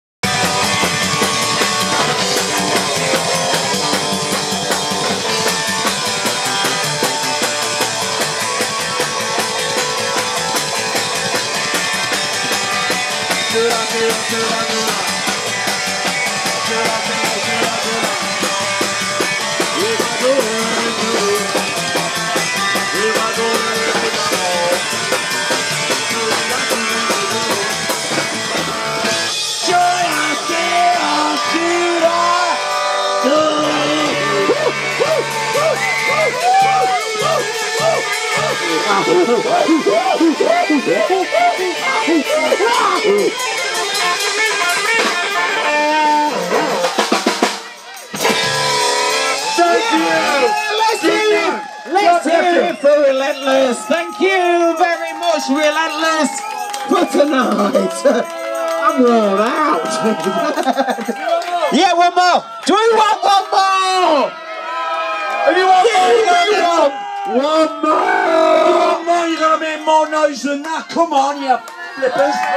JONFEST 2, 10th August 2019 (Clip 13)
13th short video clip from JonFest 2, Celebration of Jon's Bett's  50th birthday.  live music, DJ’s, food, drink. Couple of set from Relentless. Thanks for the invite. Great day had by all. More clips to follow on my YouTube channel.